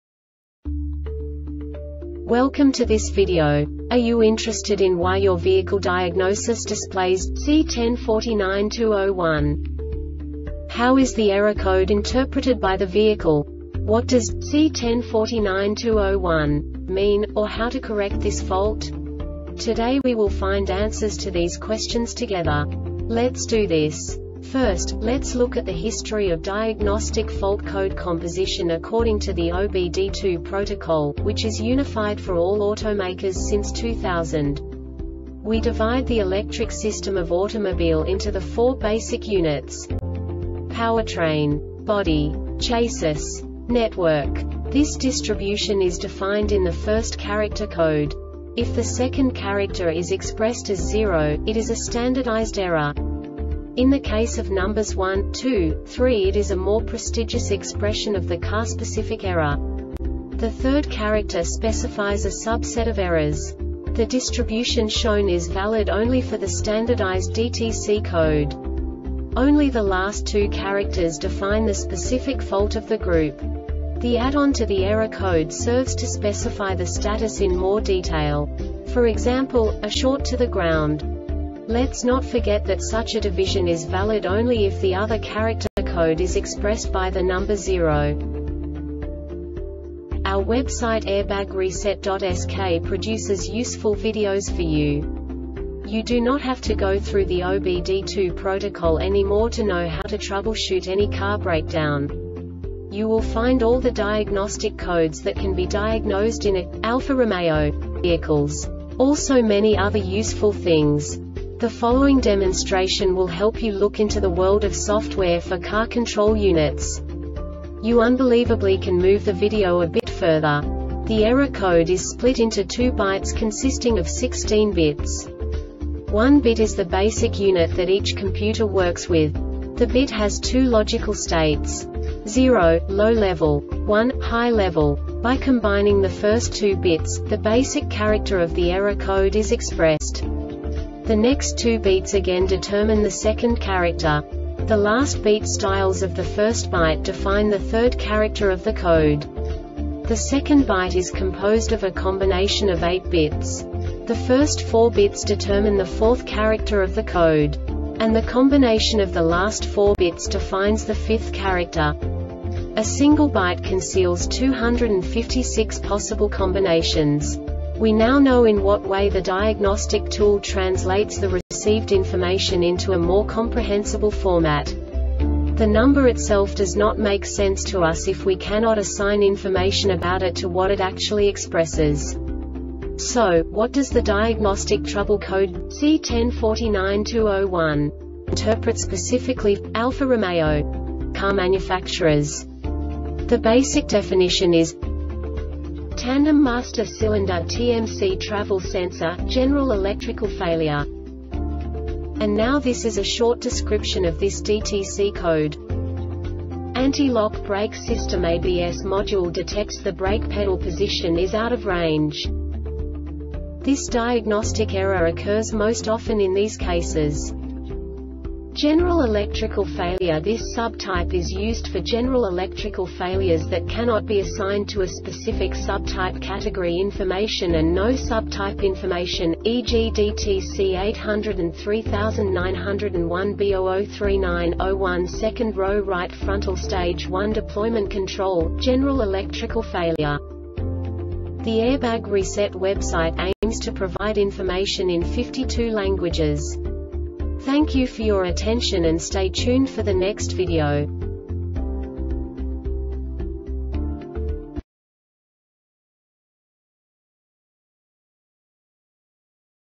DTC Alfa-Romeo C1049-01 Short Explanation
Contents:
0:21 Basic DTC analysis according to OBD2 protocol standard.
1:48 Insight into programming
2:58 A diagnostically understandable description of the Diagnostic Trouble Code
3:05 Basic error definition

Make:
Alfa Romeo
Code:
C1049-01
Definition:
Tandem Master Cylinder (Tmc) Travel Sensor - General Electrical Failure
Description:
Anti-lock Brake System (ABS) Module detects the brake pedal position is out of range.